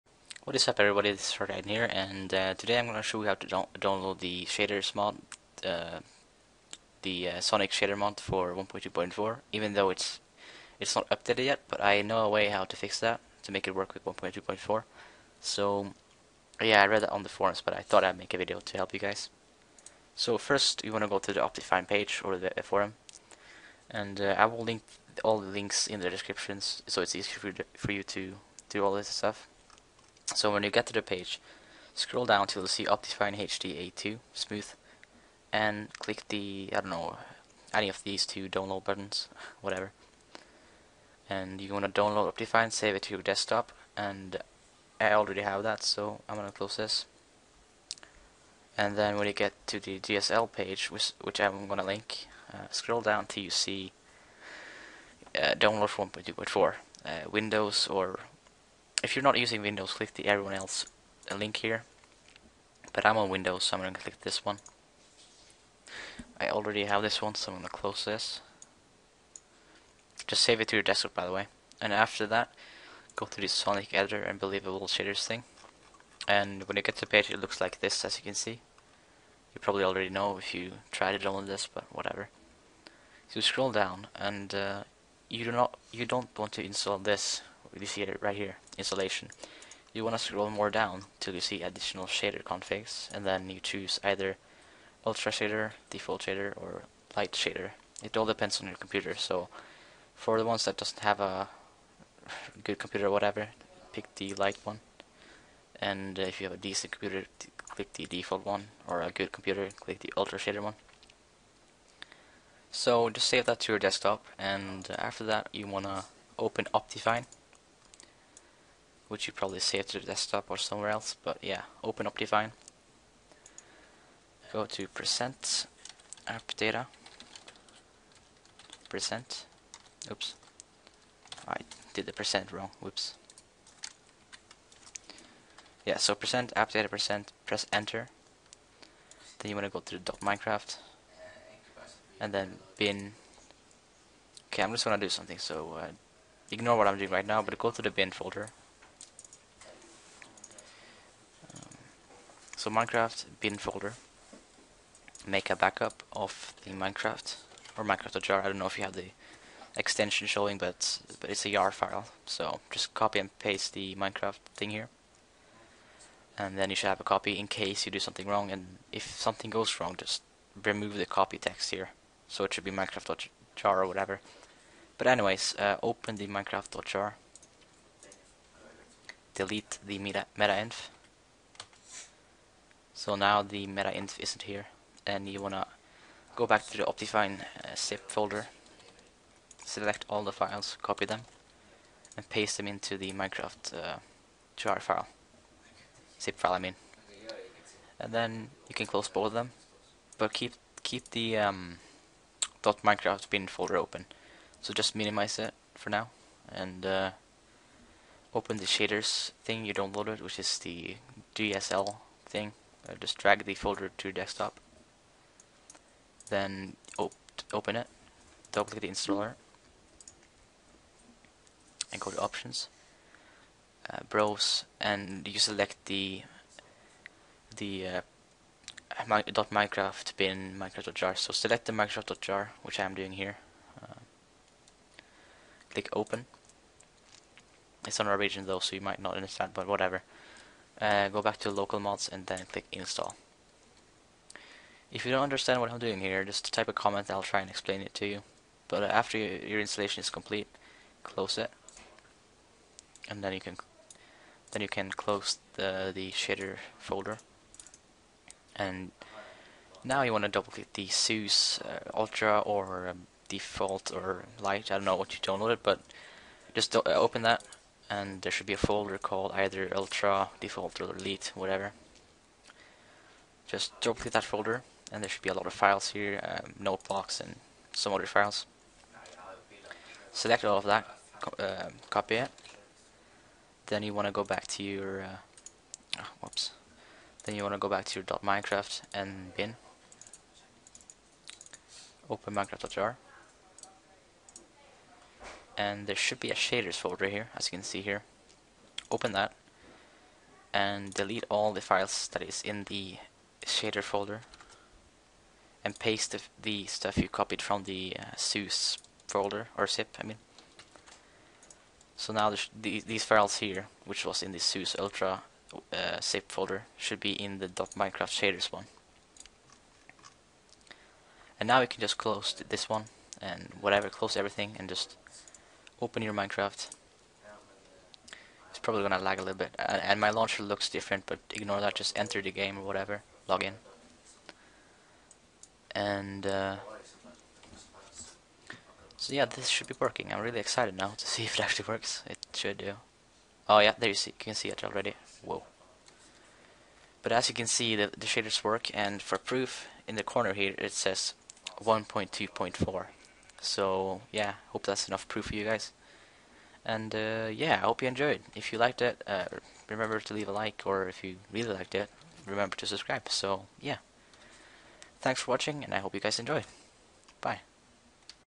Sonic Ether Unbelievable Shader mod - How to install it for 1.2.4 even though it's for 1.2.3!
One of my old videos.
Check the newer ones for better quality!